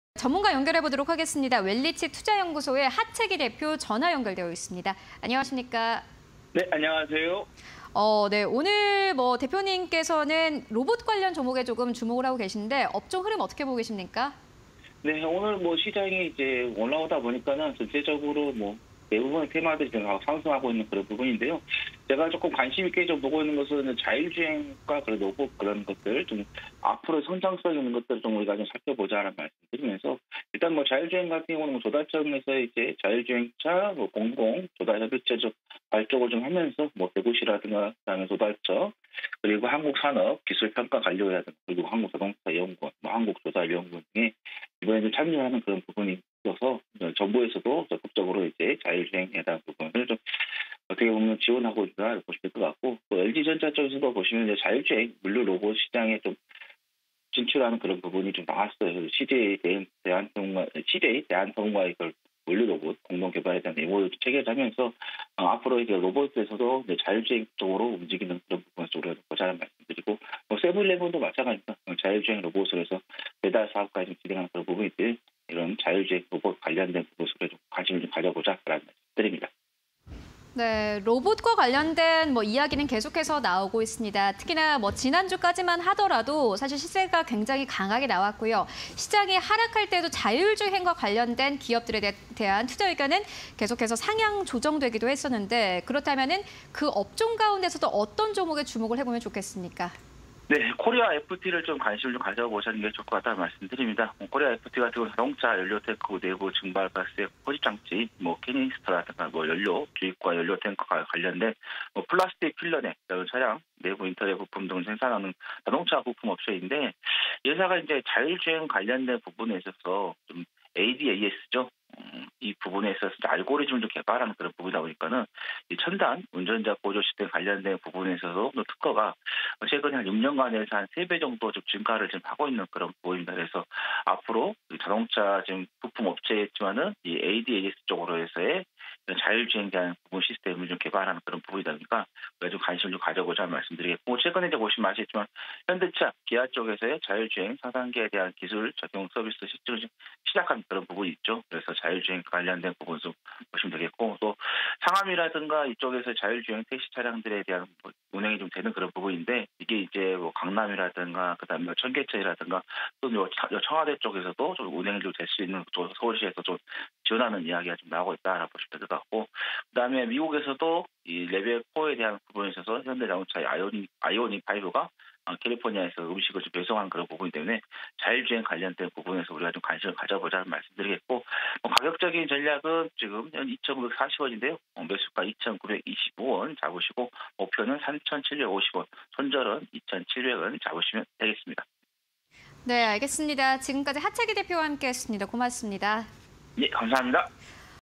코리아에프티(123410)... 머신러닝 기반 ADAS 소프트웨어 알고리즘 개발_종목 TALK (20220616)
Q. 오늘 관심 업종 흐름은?
- 서비스 로봇 관련 업종에 주목
- 조달청, '자율주행차 공공조달협의체' 발족
- LG전자, 자율주행 물류 로봇 시장 본격 진출
- 세븐일레븐, 자율주행 로봇으로 '도심 배달' 사업

코리아에프티(123410)
- 캐니스터, 필러넥 등 자동차 내부 부품 생산
- 중국·인도·폴란드 등 생산 법인 보유
- 머신러닝 기반 ADAS 소프트웨어 알고리즘 개발
- 첨단 운전자 보조 시스템 관련 특허 출원 6년 간 3배 증가
- 현대차·기아 자율주행 4단계 기술 적용 서비스 시작
- 현대차 아이오닉5, 美 캘리포니아서 음식 배송